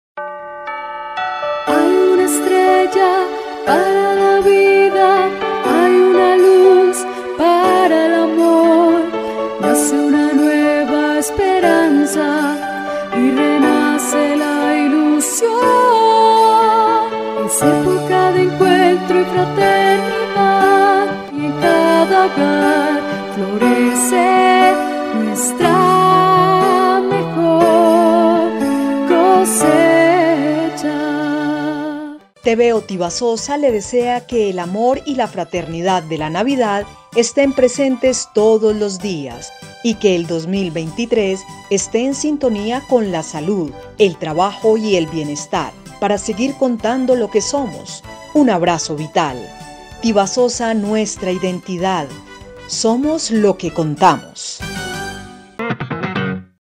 TVO TIBASOSA | MENSAJE NAVIDAD 2022
TVO TIBASOSA le desea que el amor y la fraternidad de la navidad estén presentes todos los días y que el 2023 esté en sintonía con la salud, el trabajo y el bienestar  para seguir contando lo que somos. Un abrazo vital. Tibasosa nuestra identidad, somos lo que contamos!